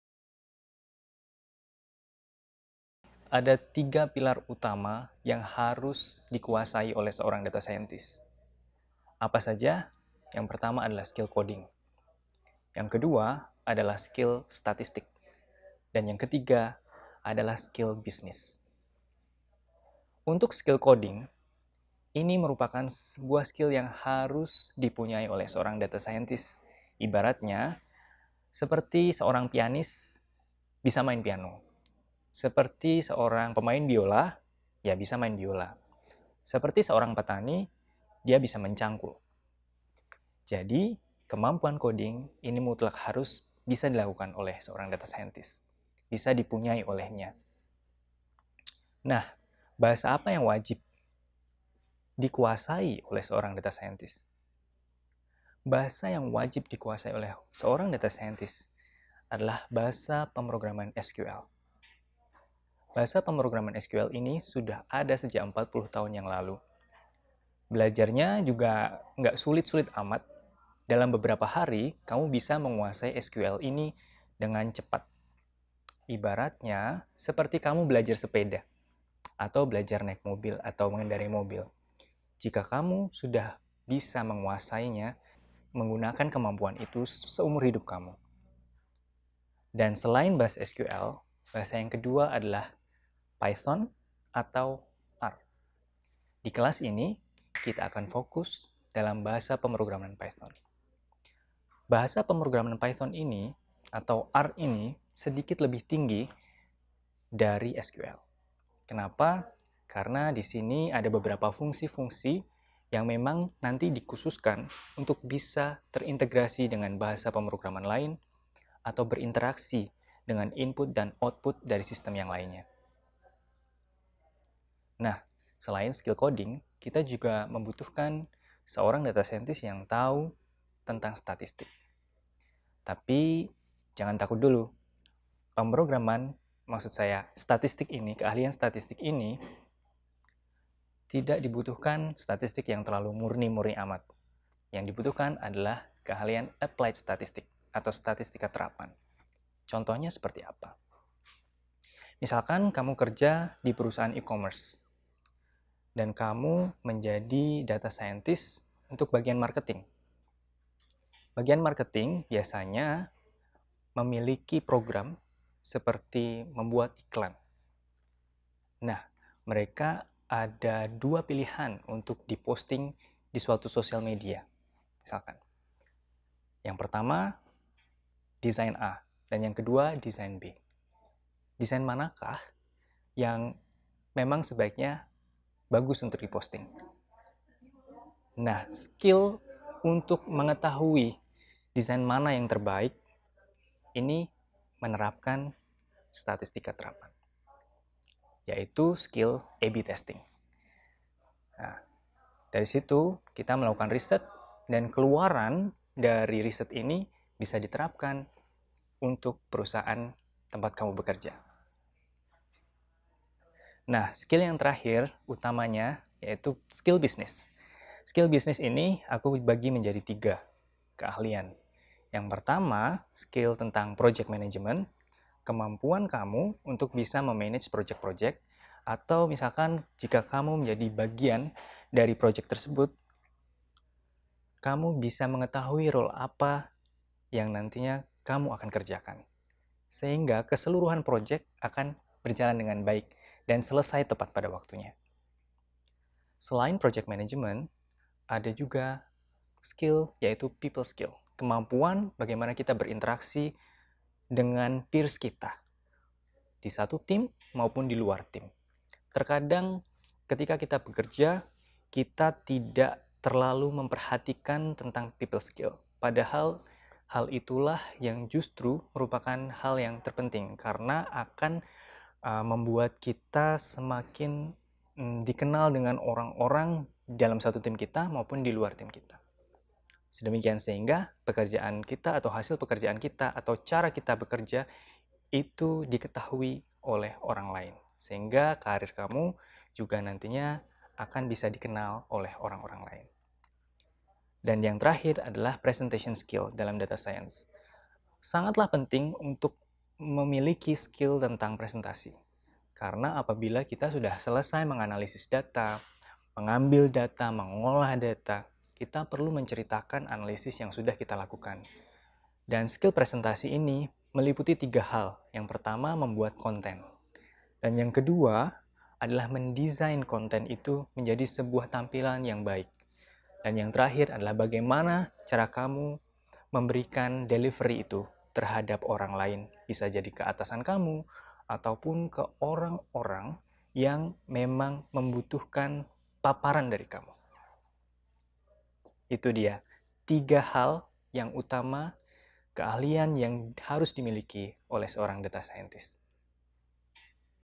Tiga Pilar Data Science
Halo, Sobat Data!

Berikut adalah cuplikan kelas DataScienceFoundation yang bisa kamu akses dan pelajari dengan secara fleksibel dari sisi waktu dan kecepatan belajar (at your own pace!)

Kunjungi KelasOnlineIYKRA di untuk mempelajari selengkapnya

Tech Think Tank Plus adalah cuplikan video dari materi yang ada diajarkan